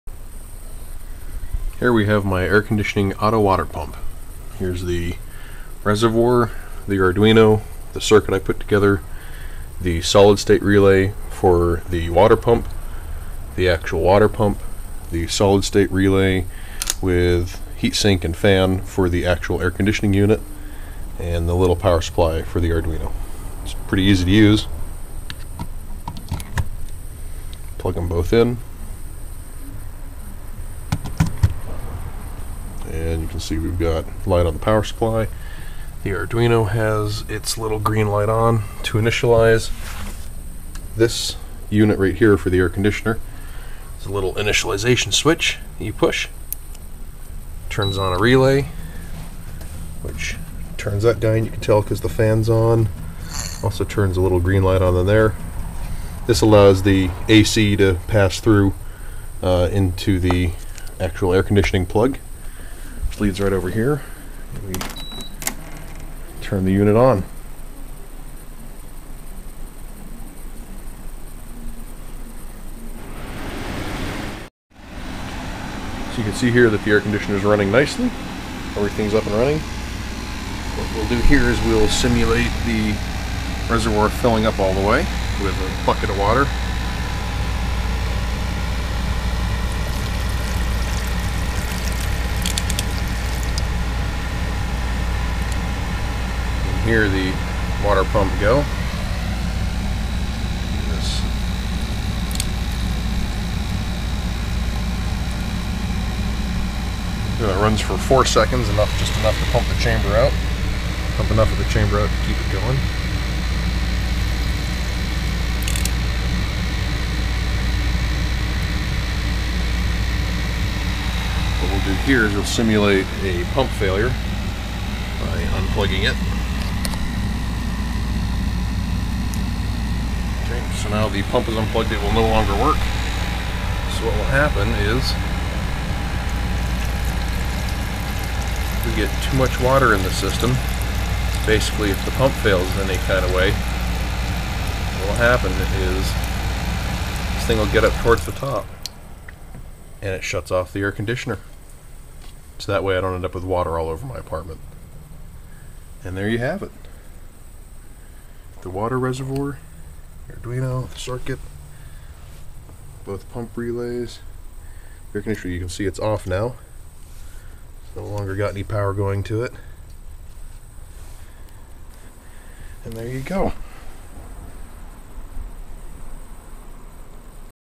Arduino A/C Water Pump
I got tired of carrying a bucket of water to the bathroom everyday, so I built an Arduino based water pump!